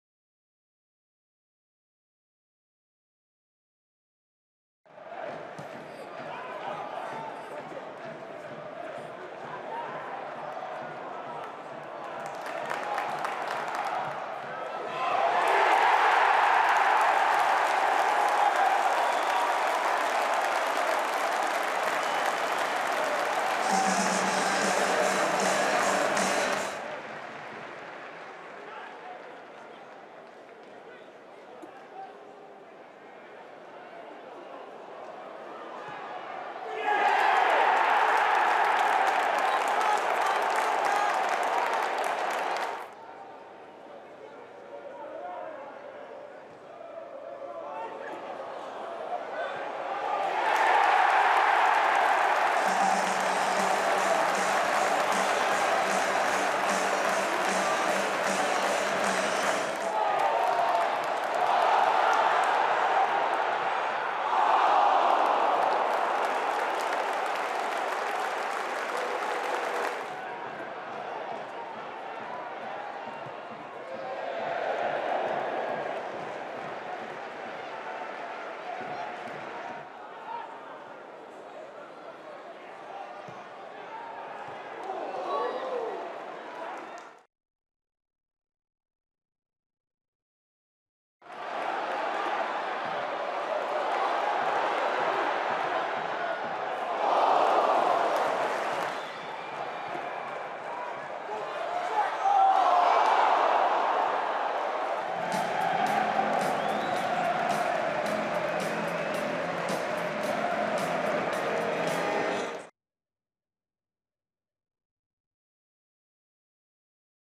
HIGHLIGHTS: MK Dons 2-1 Lincoln City
Highlights from the Sky Bet League One fixture between MK Dons and Lincoln City at Stadium MK on Tuesday 20 August 2019.

Goals: Callum Brittain (MK Dons), Tyler Walker (Lincoln City), George Williams (MK Dons)